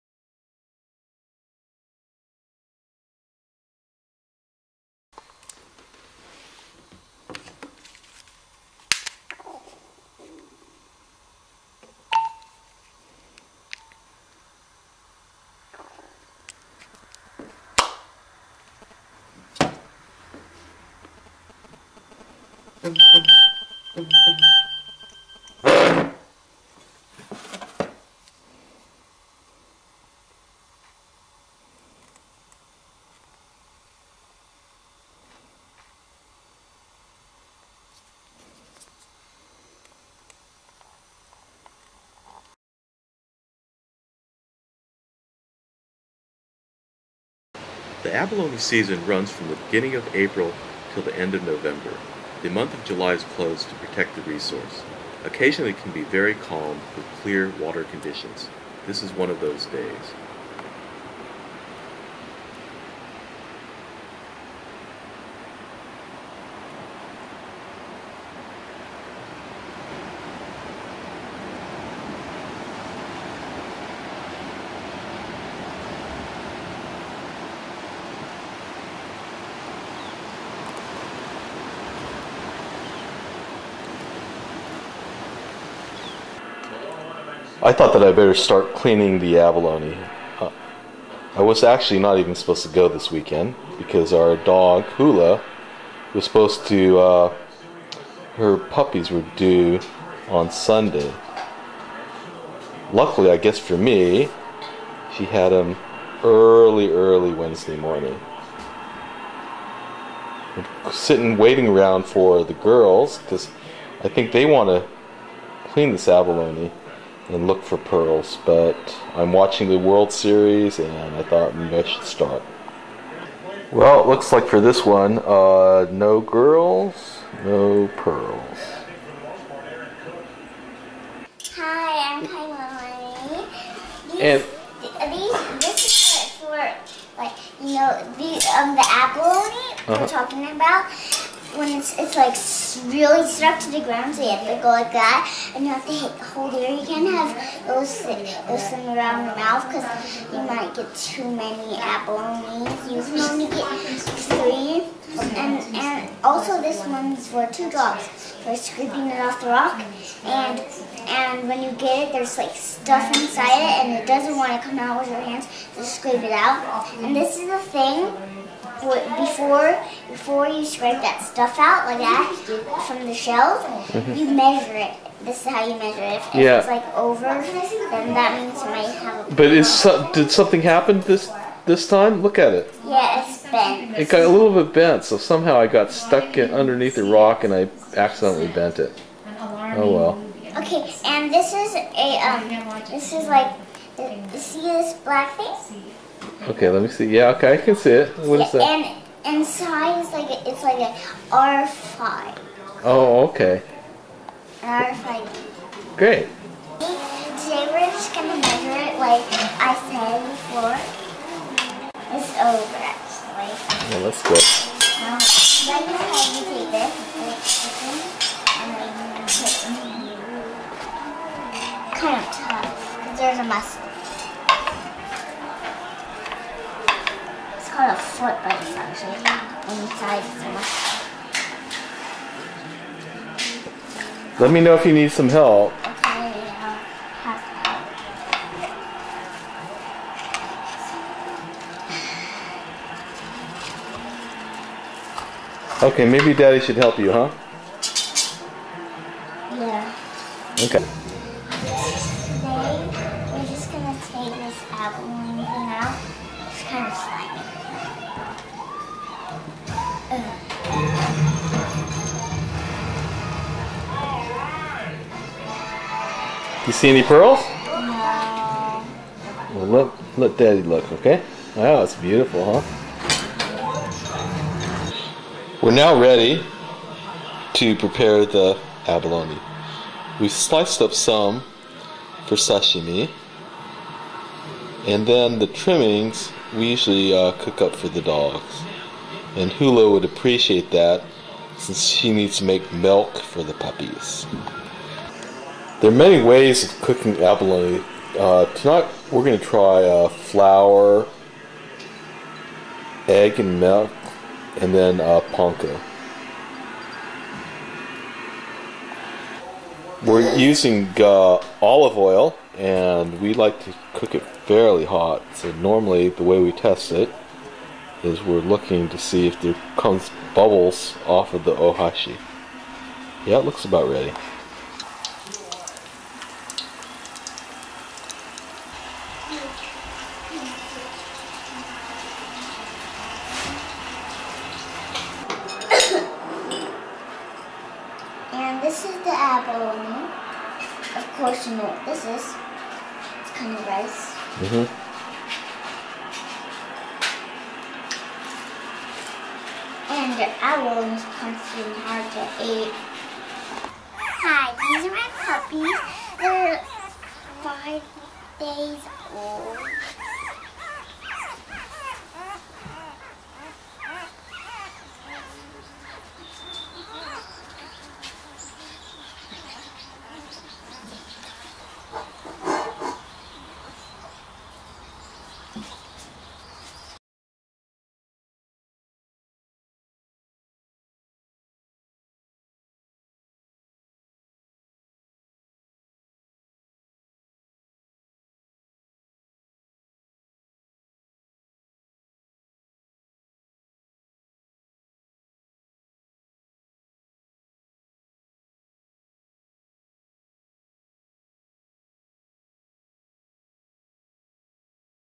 Red Abalone: Memories from the past, E012000701269E14
We have attached a mifare RFID tag to an abalone iron.  We are using cell phones to read and replay videos of our outdoor adventures.